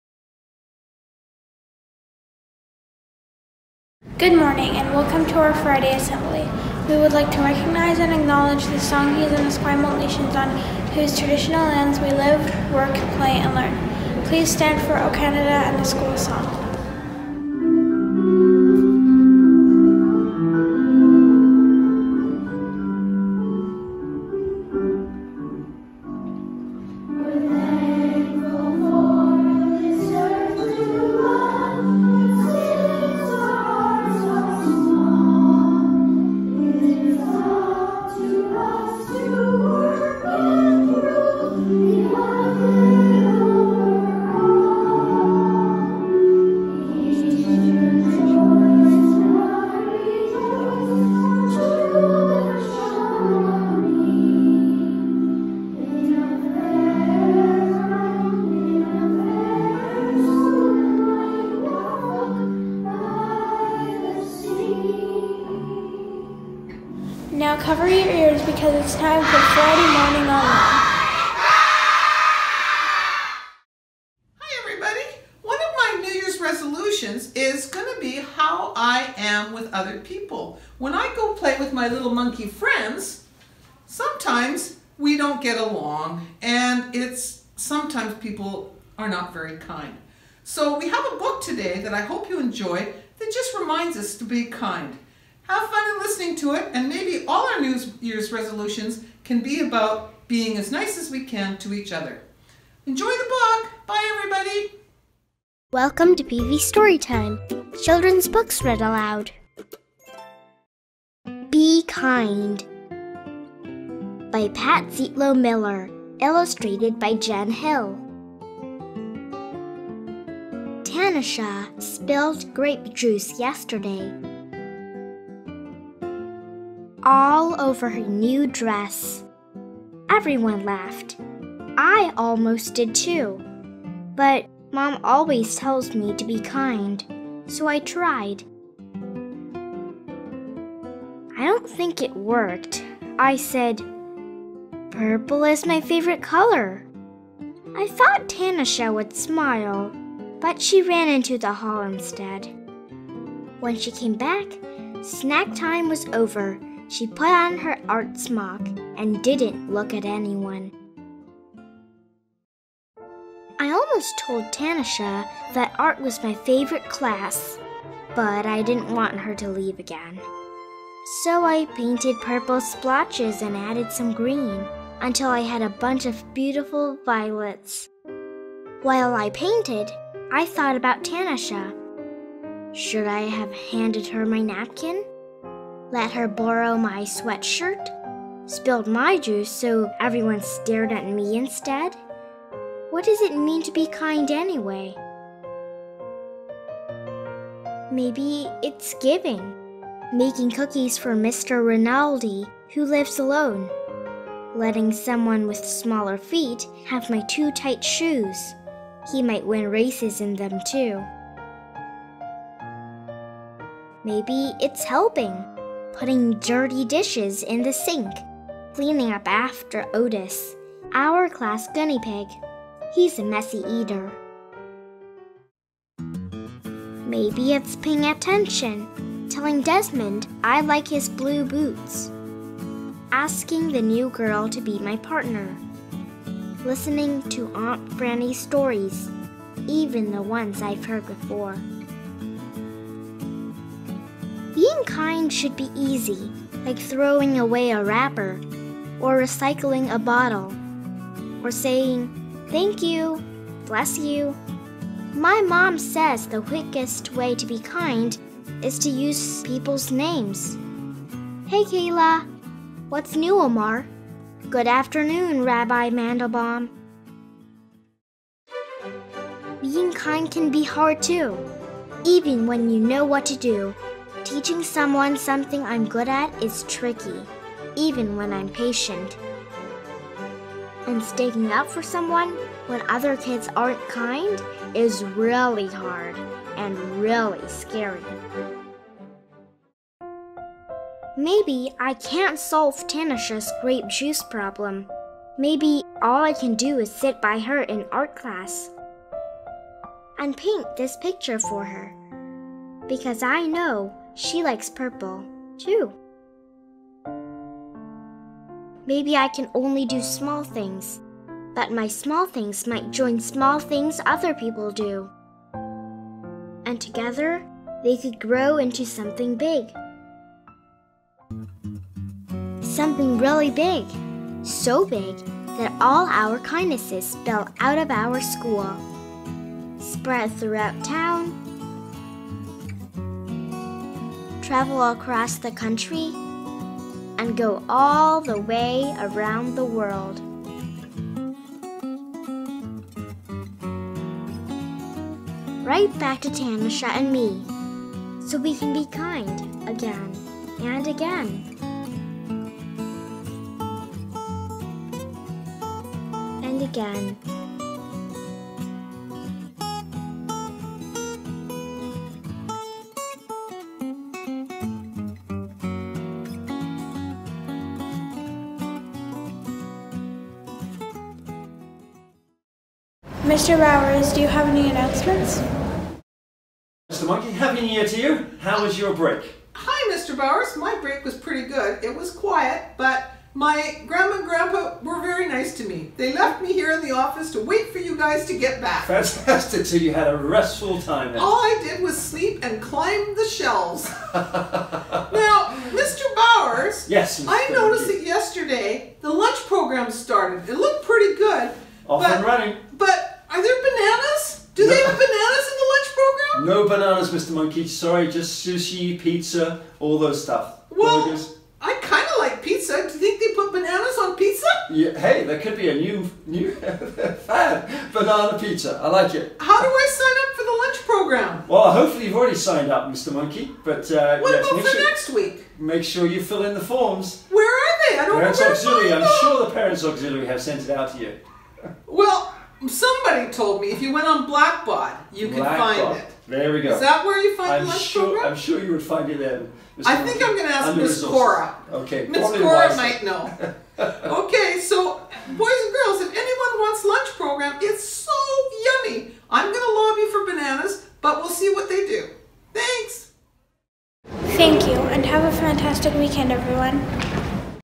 Friday Morning Online: January 8, 2021
This week's Friday Morning Online Assembly is about being kind to one another as we ring in the New Year! It also features lunch program reminders, a reading of 'Be Kind' by Pat Zietlow Miller, and more.